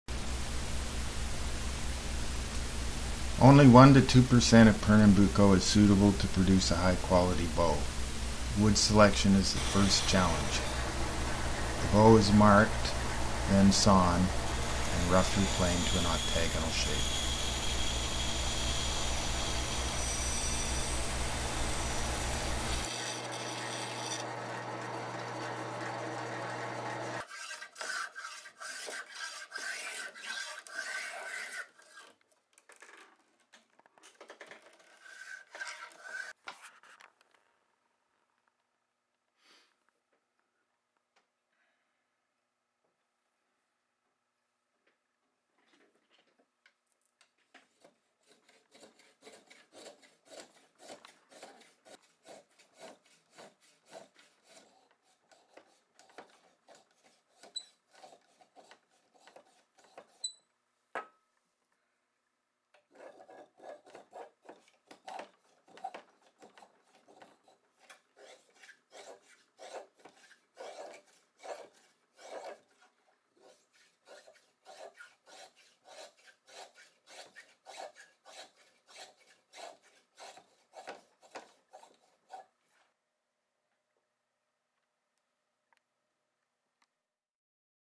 Making A Violin Bow: 1. The beginning
Making a Violin Bow: The Beginning
Reid Hudson takes you through the steps of making a violin bow.